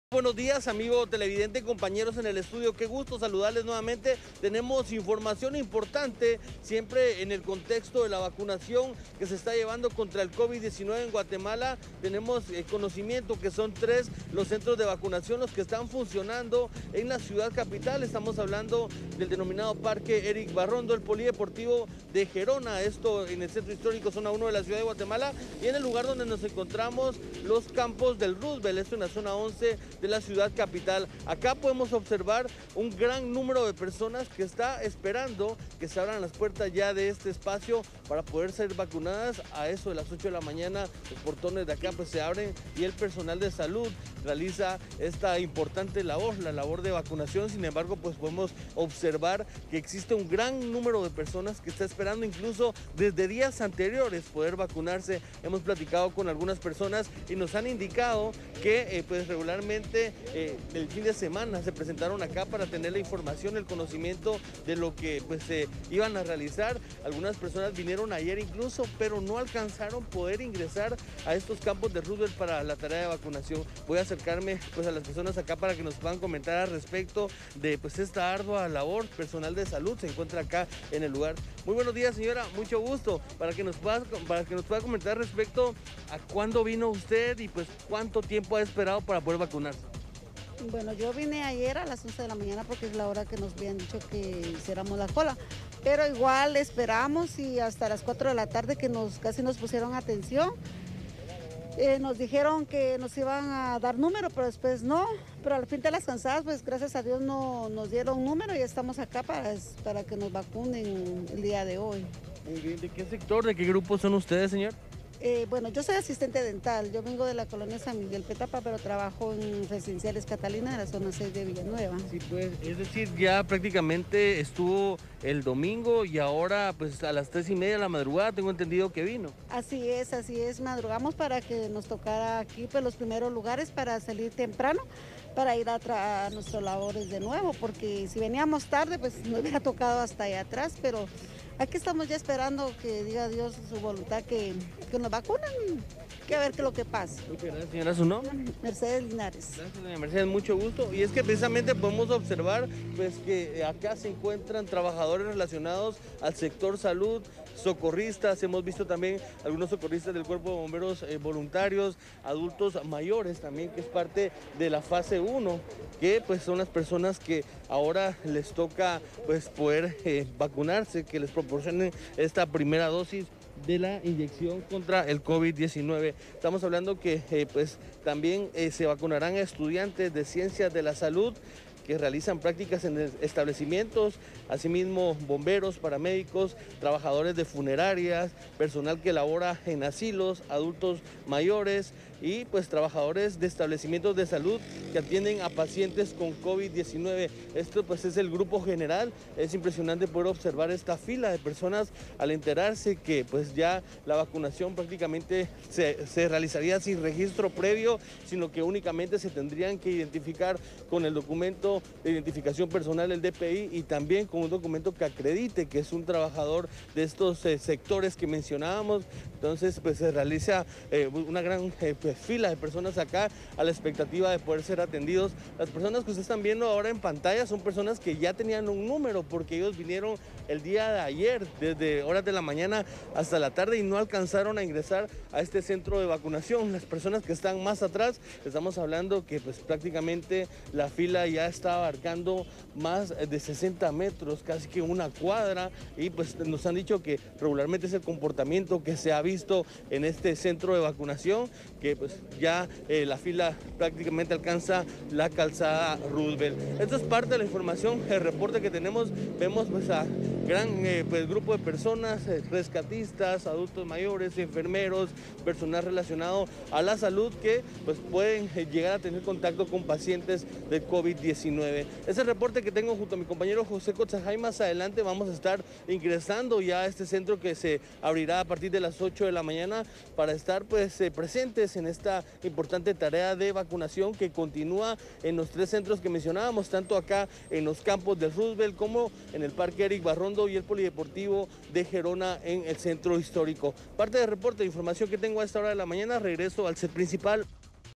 Tres centros de vacunación contra Covid 19 habilitados en Ciudad Capital
Parque Erick Barrondo, Polideportivo Gerona y el Hospital Roosevelt

Enlace desde las afueras de los campos del Roosevelt en donde está instalado un centro de vacunación, personas han realizado filas desde durante dos días para poder ser vacunados.